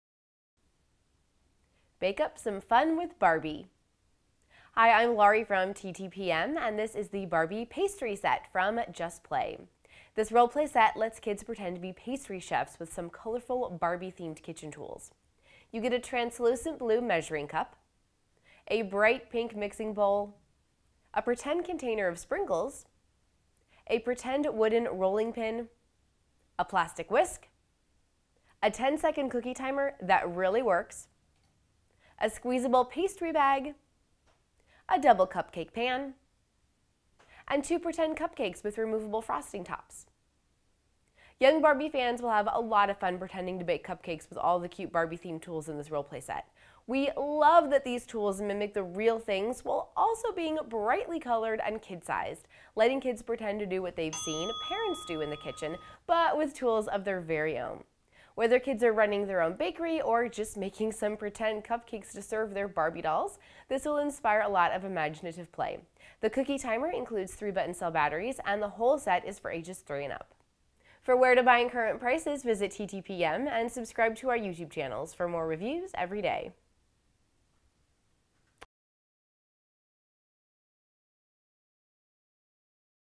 Barbie Pastry Set from Just Play
Bake fun with Barbie and the Barbie Pastry Set! Comes with 13 play pieces, including a working cookie timer! Cute set that will inspire creative play! See all the fun toys in this video review! For full review and shopping info

Product Info:
Bake up some fun with Barbie and the Barbie Pastry Set. This roleplay set lets kids pretend to be pastry chefs with some colorful Barbie-themed kitchen tools. You get a translucent blue measuring cup, a bright pink mixing bowl, a pretend container of sprinkles, a pretend wooden rolling pin, a plastic whisk, a 10-second cookie timer that really works, a squeezable pastry bag, a crackable egg, a double cupcake pan, and two pretend cupcakes with removable frosting tops.